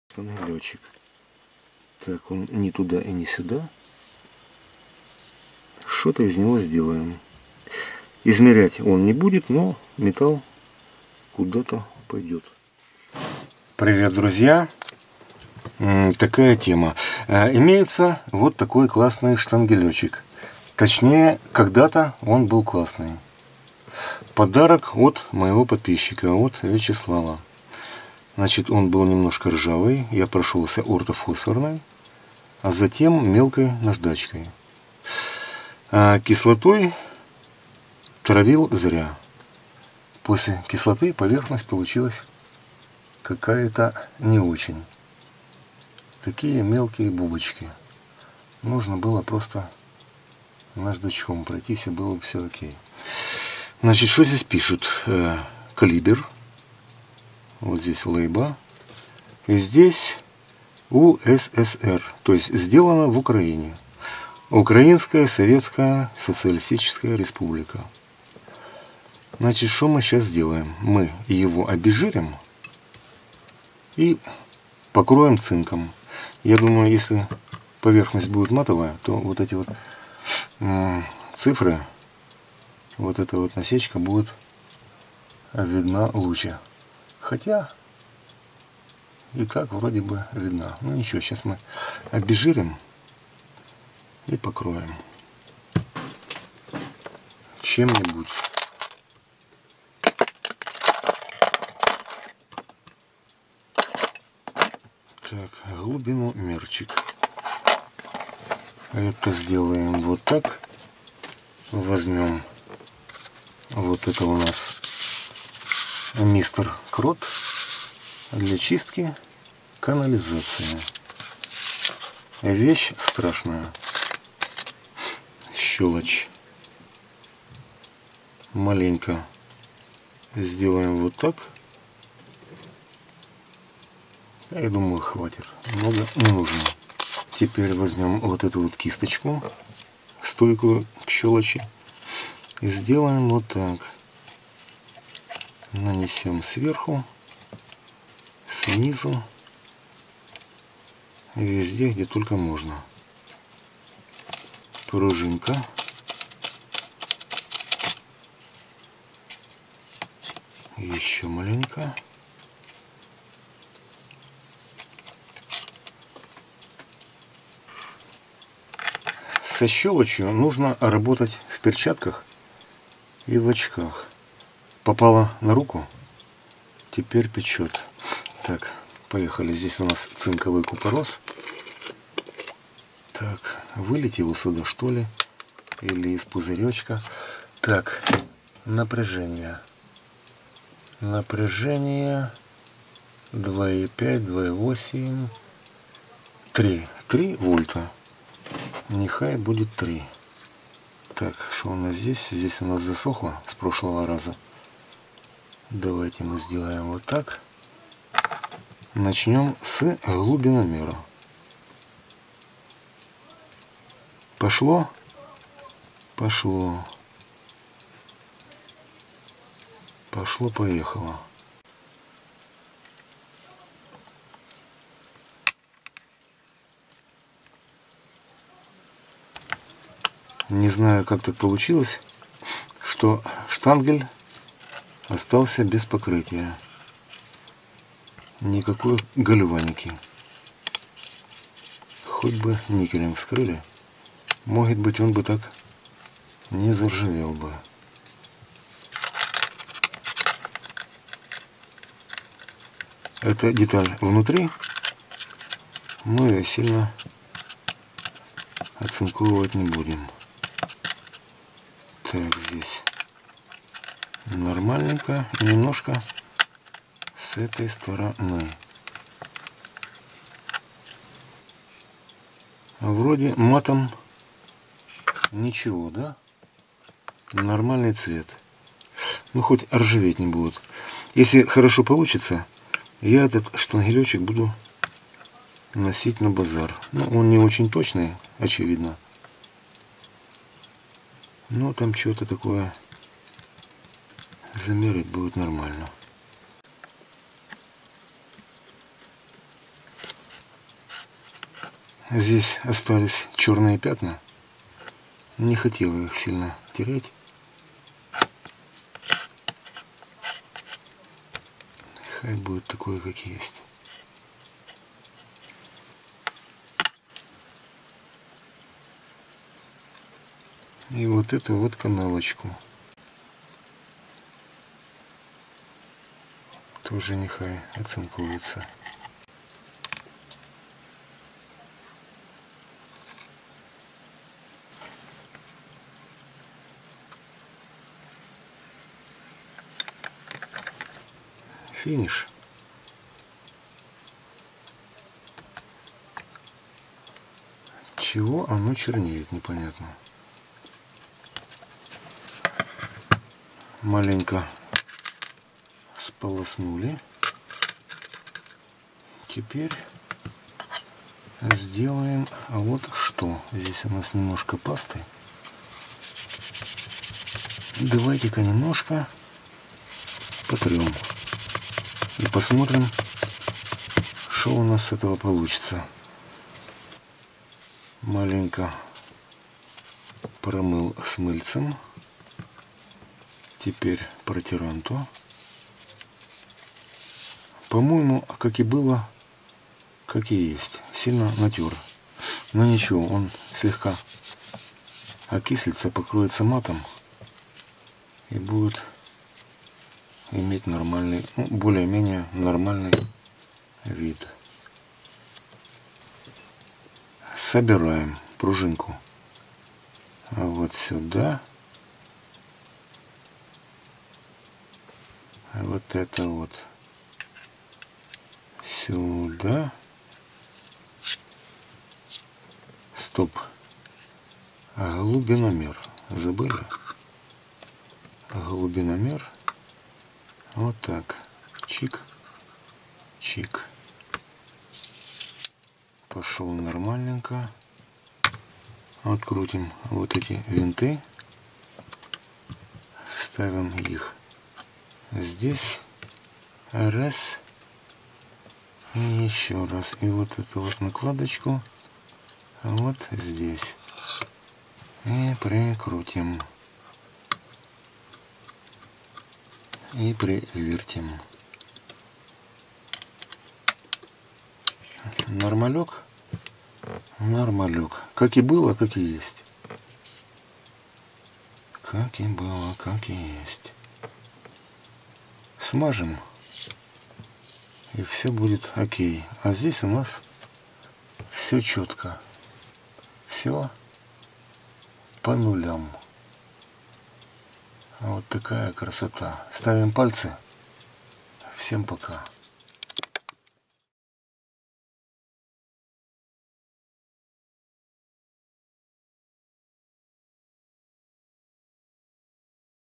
Оцинковка старого штангенциркуля DIY
Карта для поддержки канала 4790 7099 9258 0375 Но лучше отправьте ВСУ!
Сделано в УССР. Советский штангенциркуль, доводим до ума.